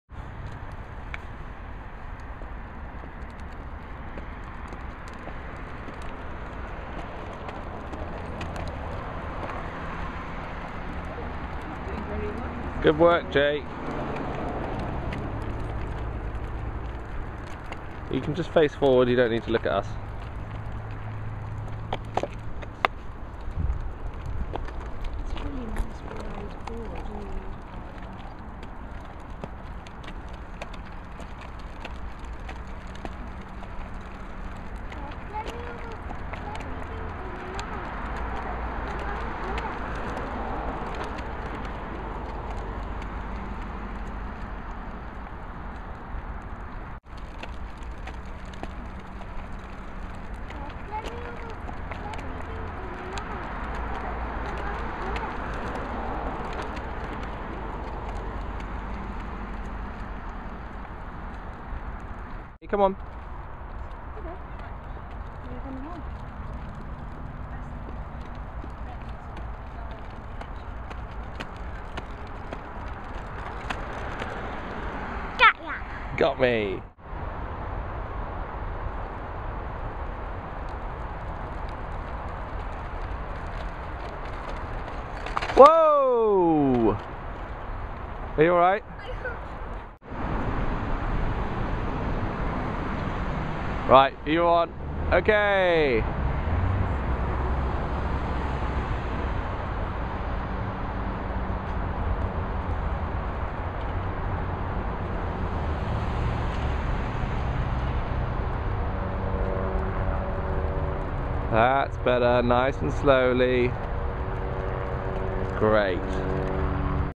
Scooter
Jake tries out his new scooter, December 2009